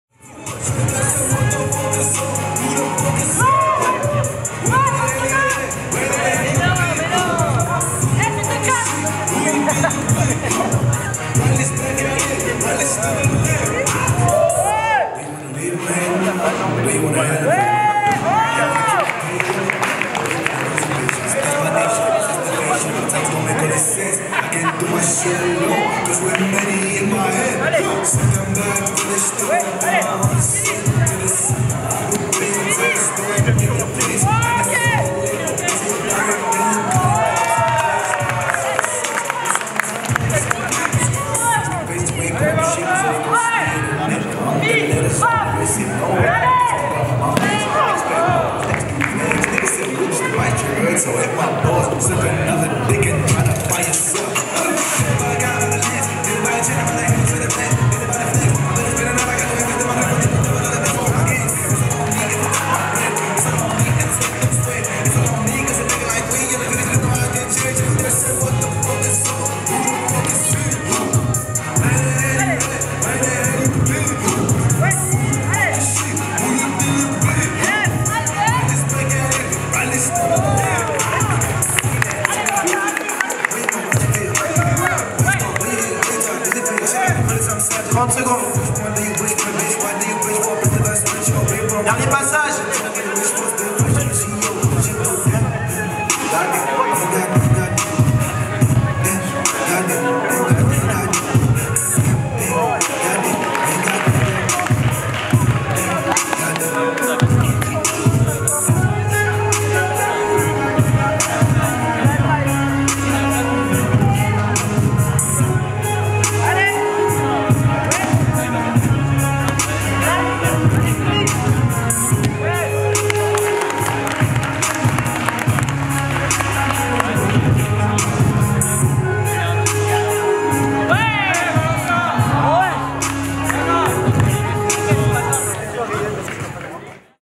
Advance Challenge XIV battle 7
L' Advance Challenge XIV de 2019: compétition nationale d'Arts Martiaux Artistiques et Acrobatiques, qui réunit les grands clubs de tricking Français et les sportifs internationaux, et où les compétiteurs s'affrontent en Battles et Katas